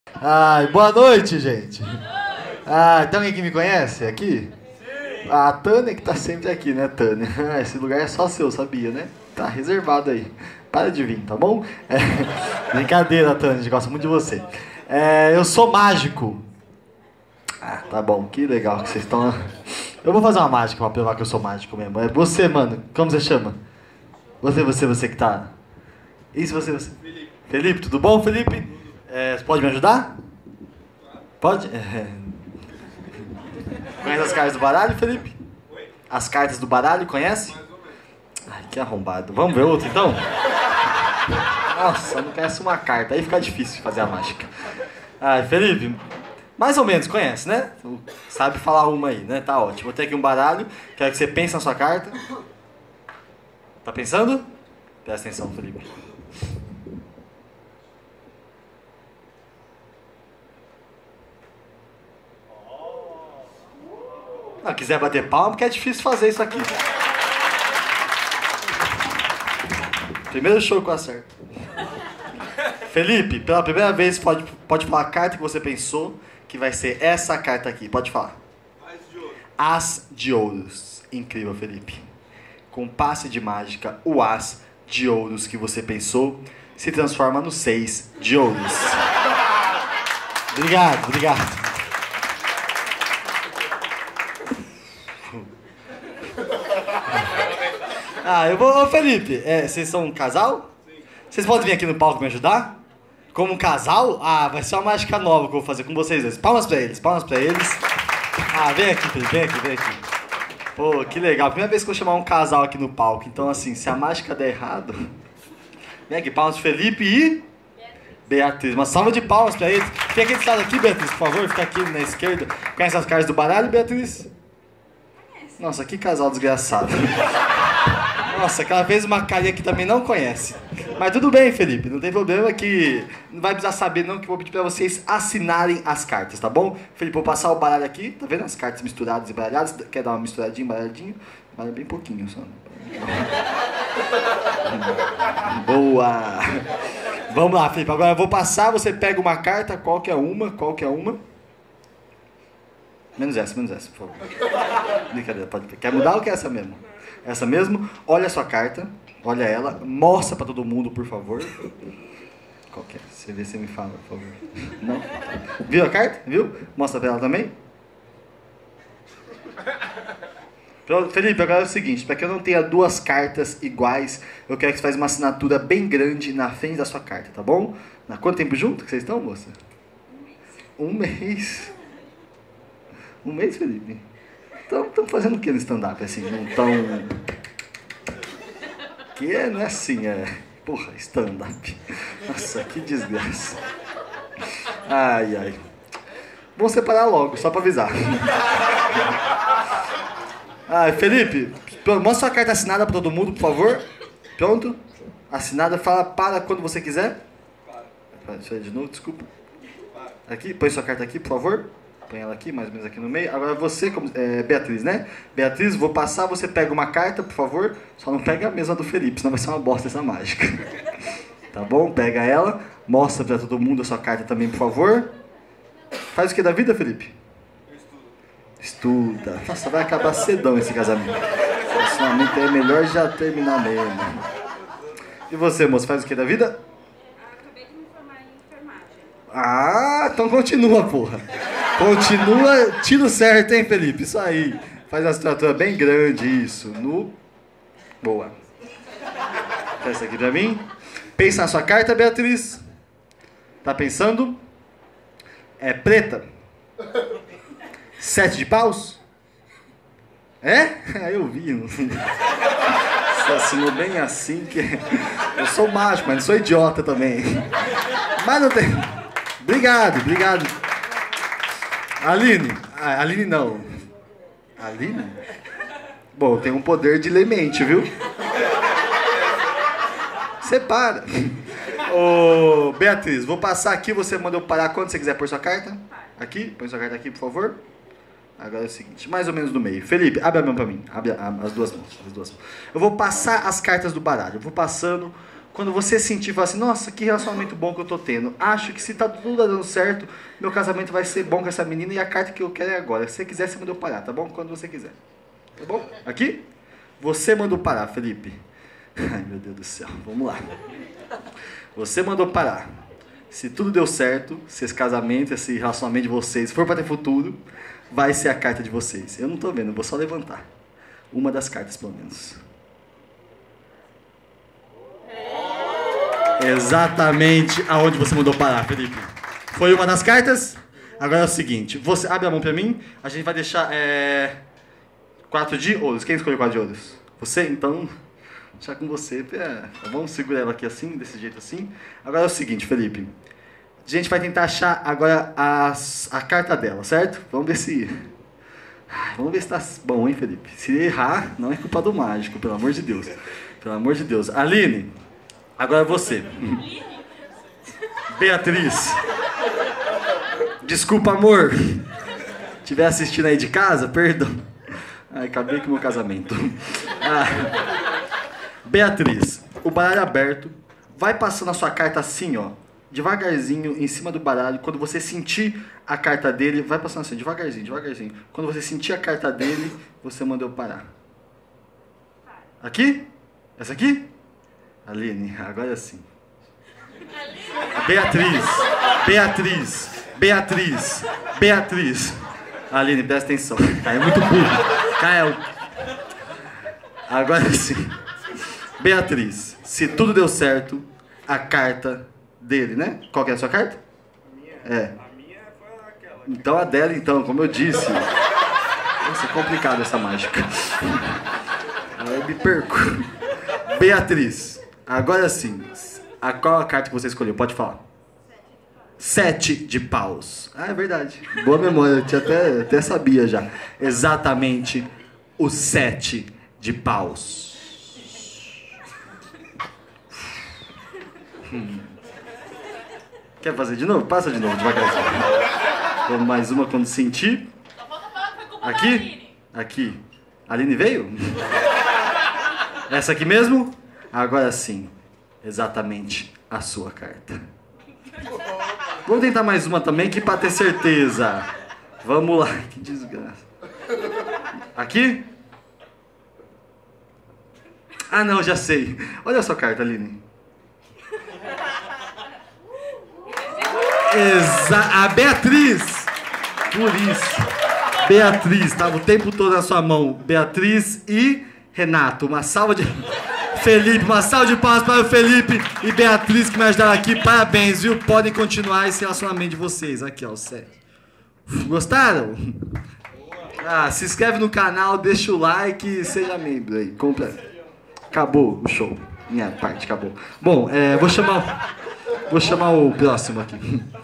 SE VCS SEPARAREM NÃO É MINHA CULPA - Stand-up Mágica
Chamei um casal que estava na Plateia para participar de uma mágica,
se a mágica der errado é melhor separar.